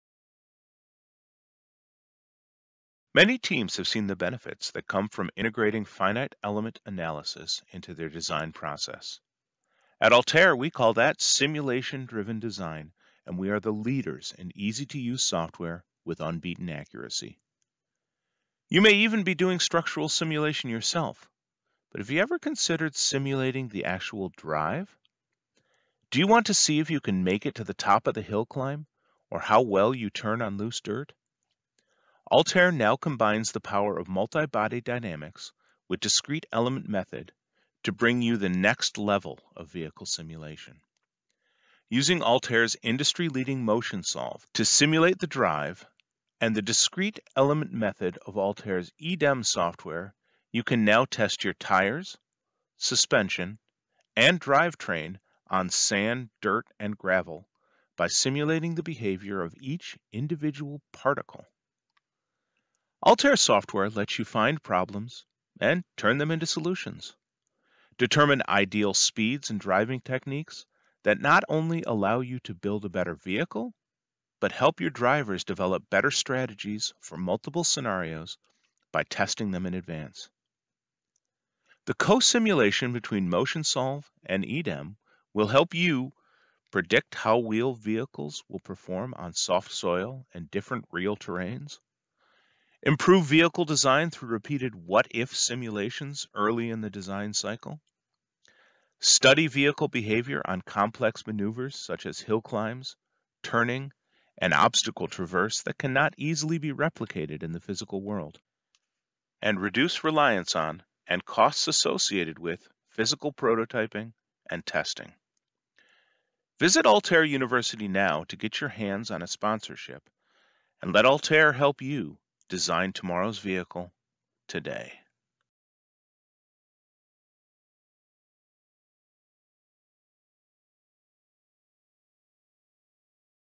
Altair EDEM Cosim for BAJA
Off-road vehicles such as the Baja student vehicles must perform in very different and challenging terrains. Altair EDEM® coupled with Altair MotionSolve™ accurately predicts the behaviour of vehicles going over soft soil, Easily perform what-if studies to understand how different vehicle designs or soil characteristics affect performance.